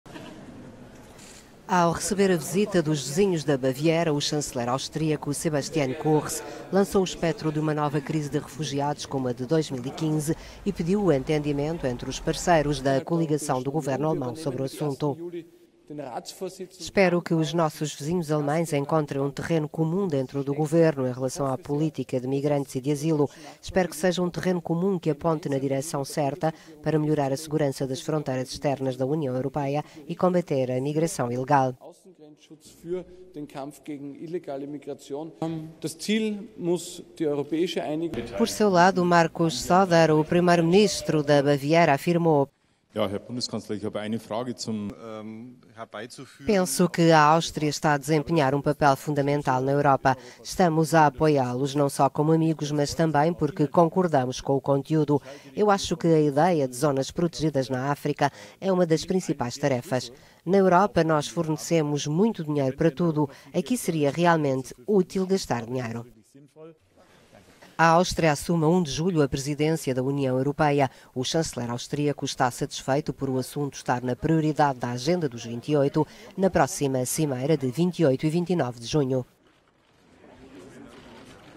Áustria e Baviera partilham política de migração
O primeiro-ministro da Baviera encontrou-se com o chanceler austríaco esta quarta-feira. O principal tema do encontro foi a política migratória europeia. Os dois líderes defendem ideias comuns sobre o assunto.…